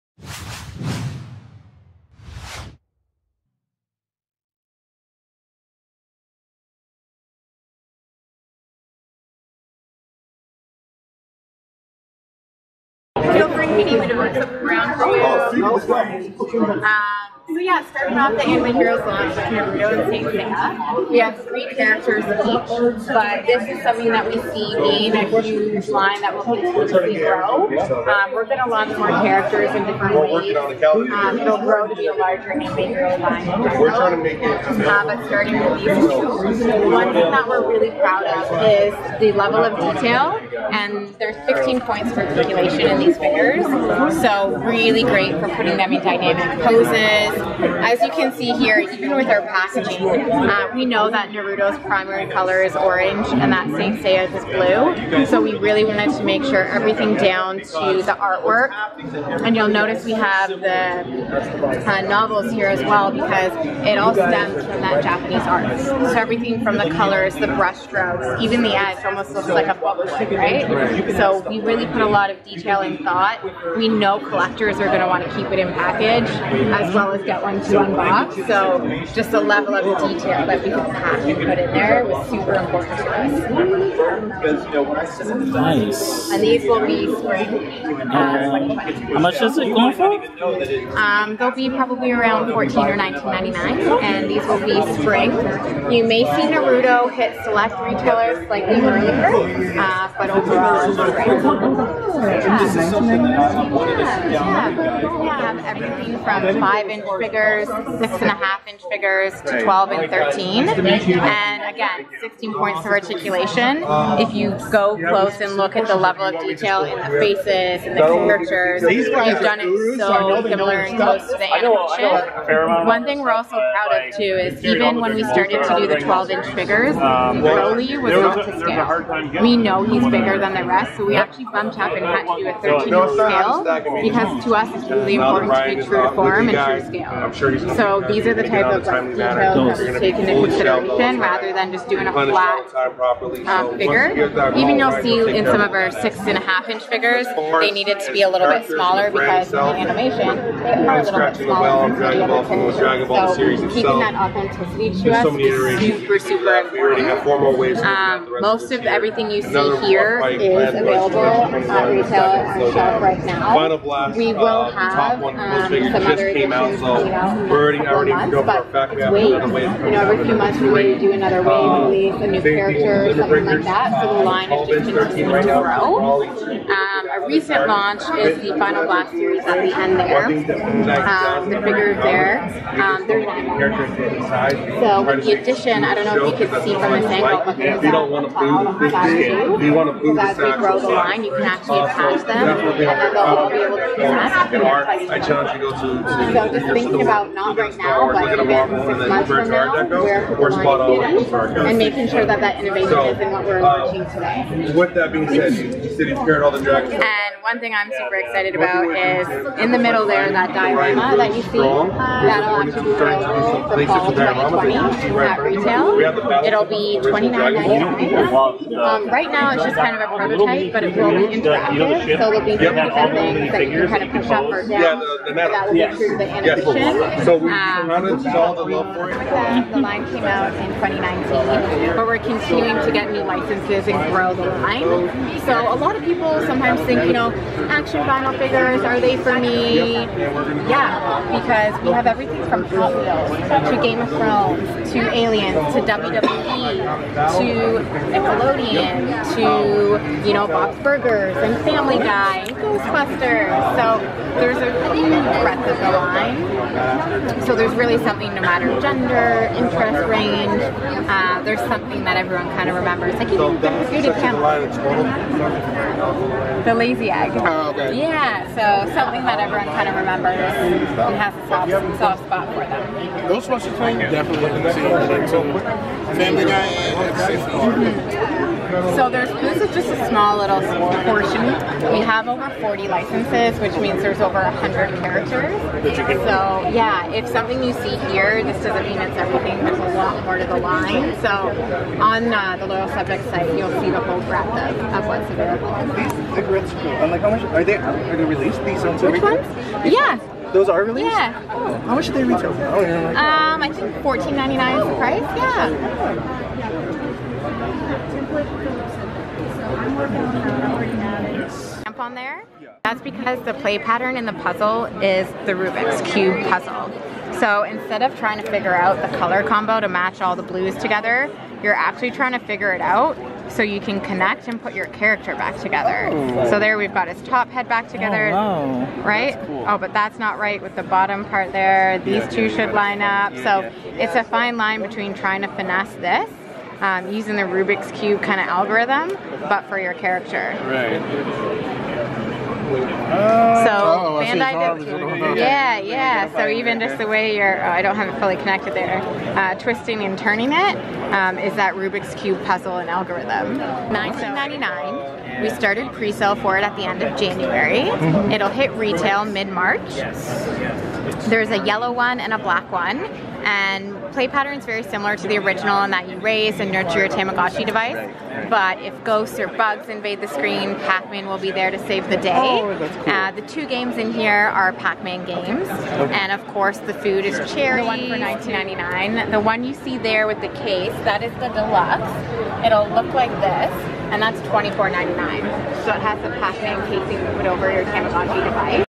Bandai (New York Toy Fair 2020)
Otakus & Geeks dropped by the Bandai booth to see the latest products from Dragonball Z, Naruto, Pac Man and more.